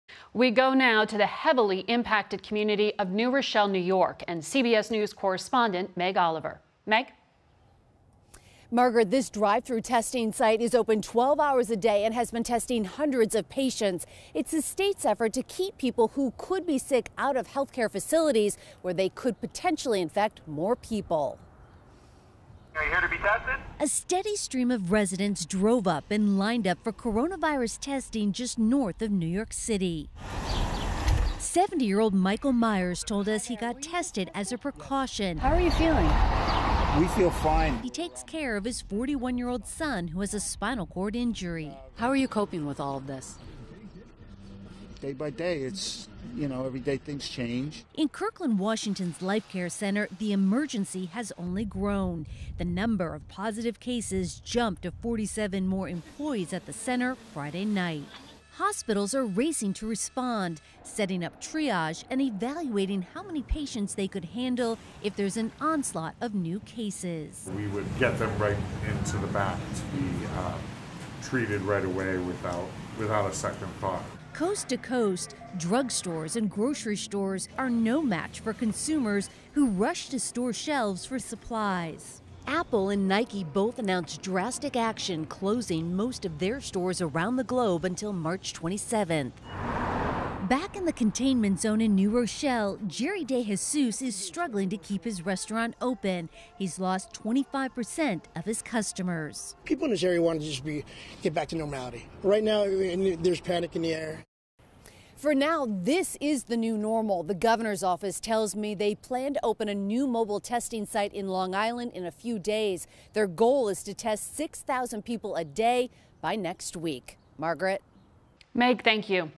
Coronavirus outbreak sparks drive-thru testing in New York
CBS News' Meg Oliver reports on the impacts the coronavirus pandemic has on New York communities.